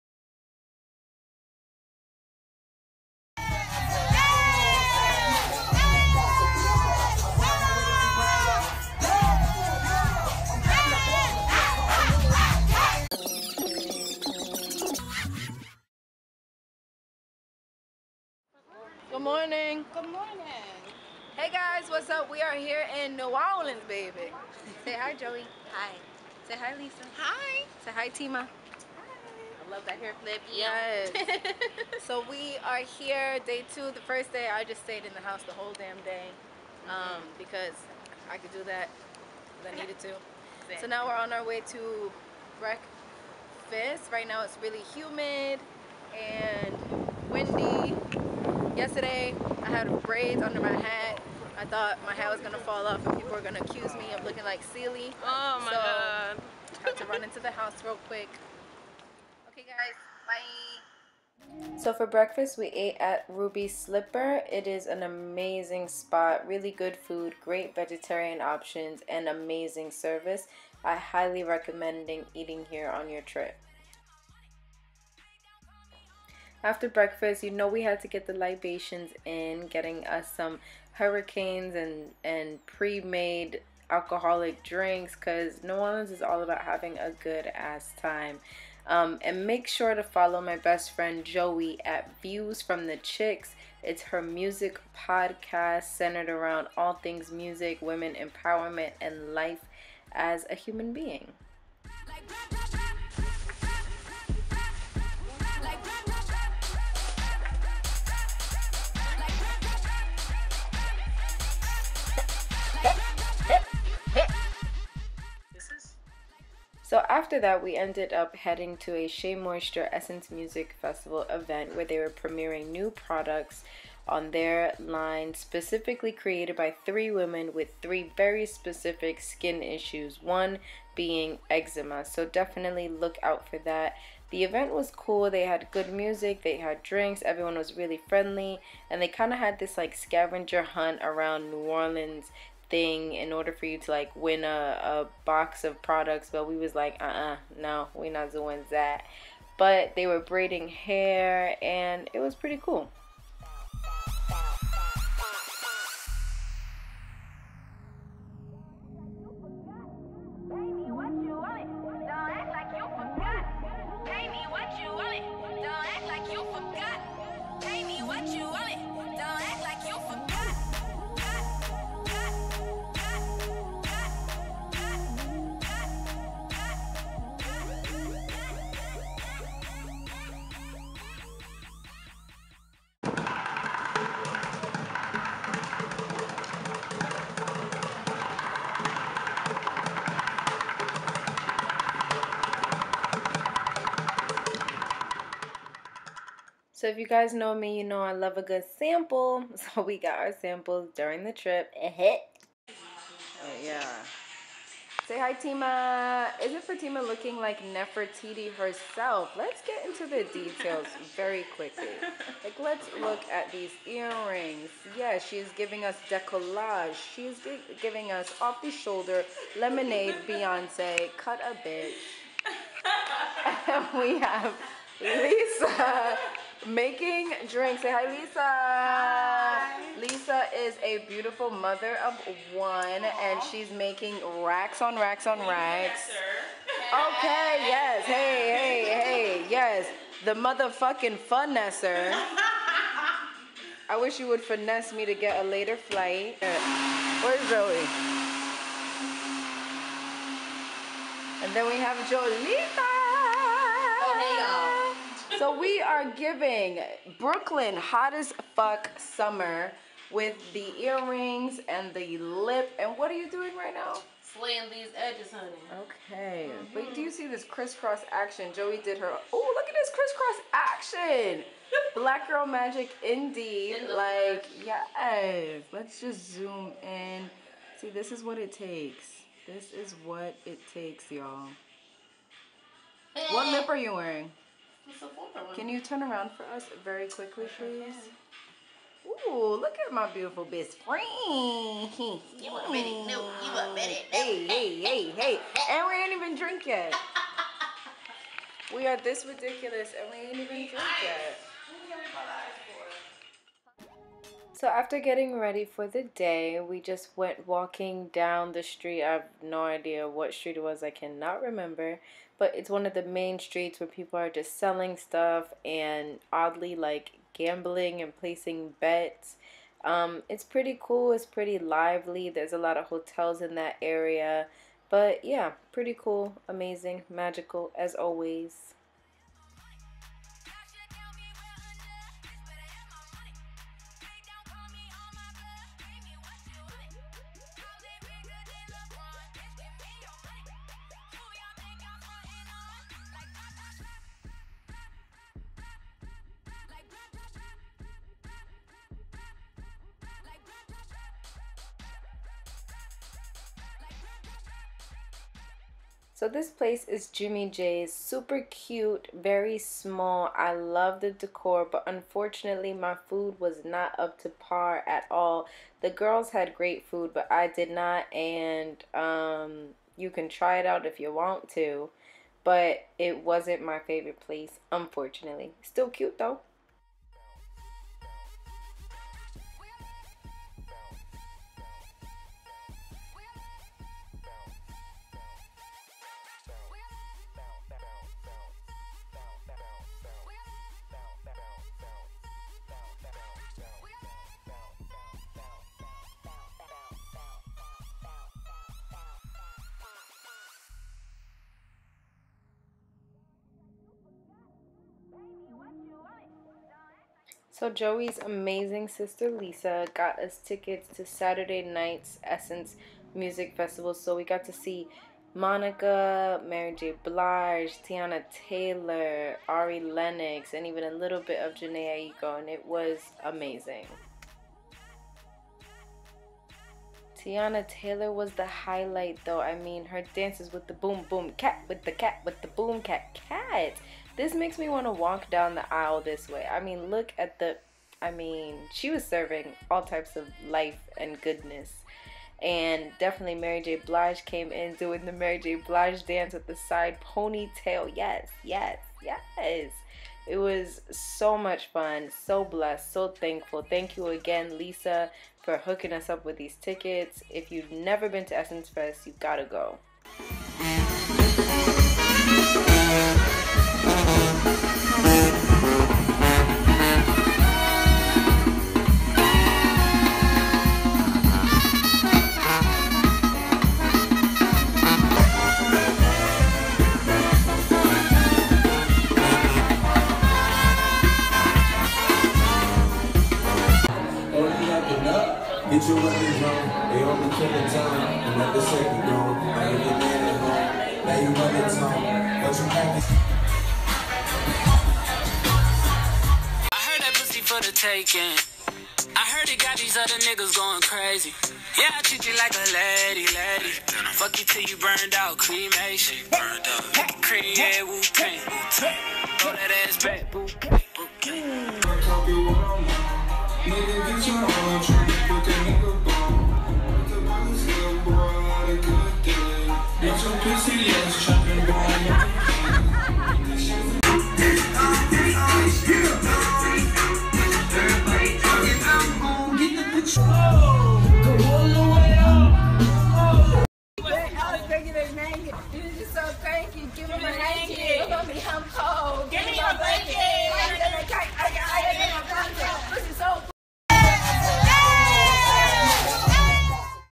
New Orleans VLOG//Essence Music Festival
Here is our beautiful magical much needed trip to new orleans. This is my 3rd year going and every year it gets better and better. This time during essence fest there was so much self love and over flow of love from one another. If you've never been to NOLA, YOU GOTS TA GO BABAY!